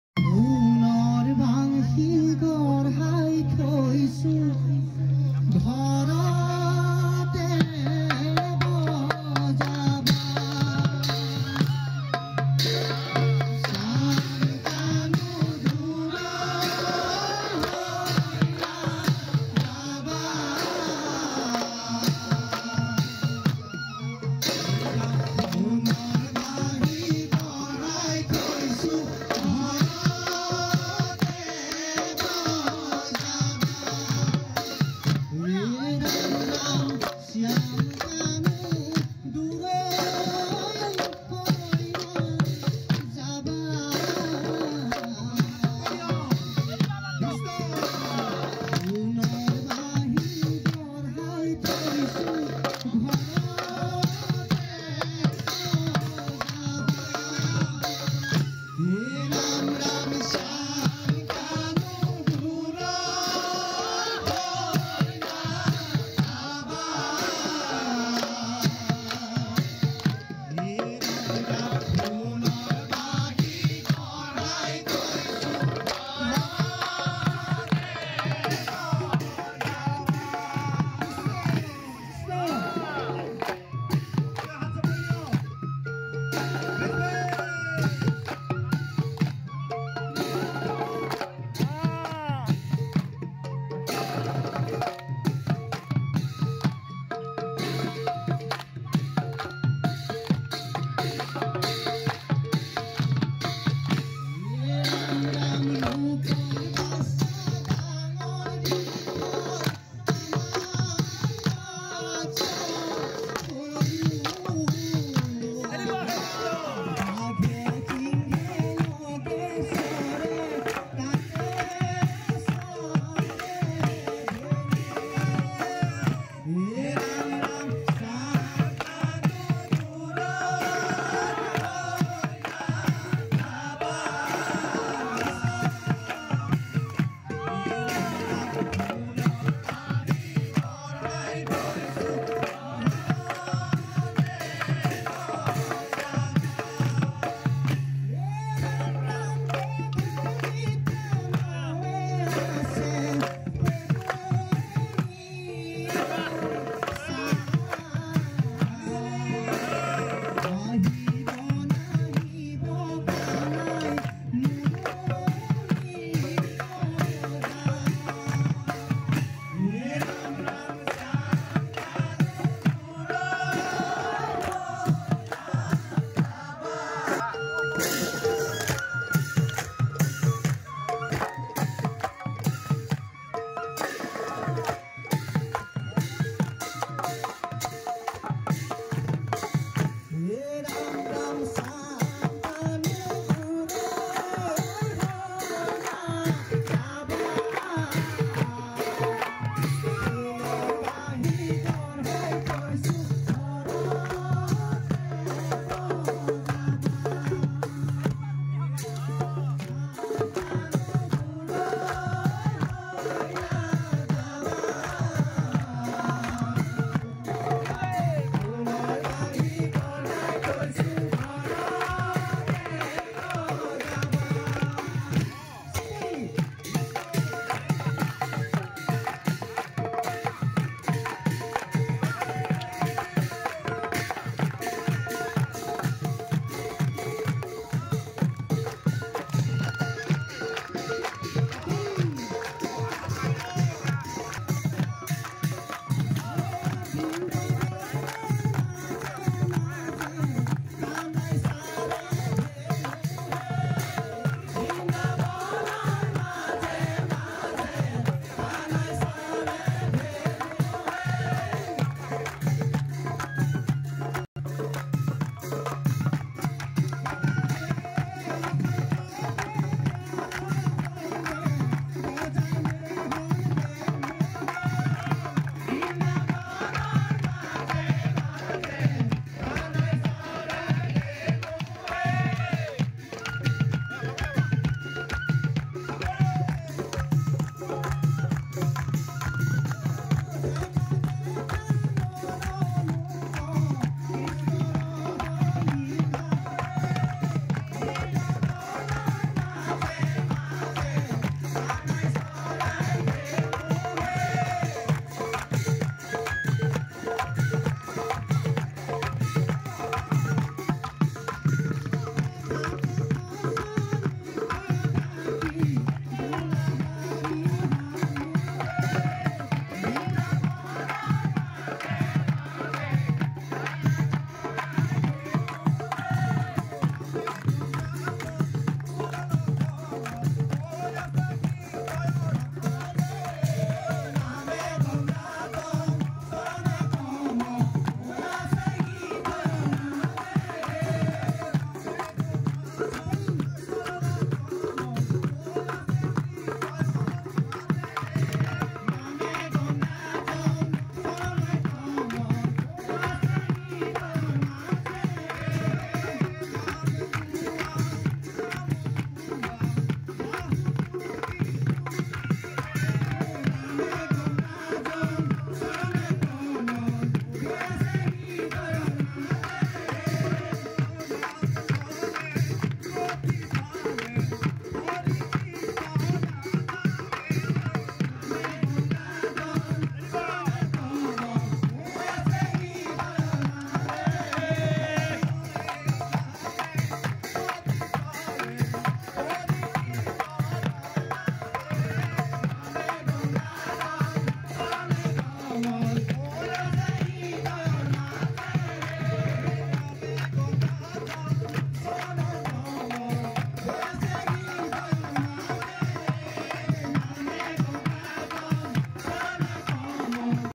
বিশেষ আকৰ্ষণ#কাতিবিহু উদযাপন কৃষিমন্ত্ৰী অতুল বৰা ডাঙৰীয়াই দিহানামত বজালে
highlights বিশেষ আকৰ্ষণ কাতিবিহু উদযাপন বোকাখাত মহকুমা কৃষি বিভাগ আৰু স্থানীয় ৰাইজৰ সহযোগত আহু গাঁও আৰু যোগীগাঁও সংযোগী পথত।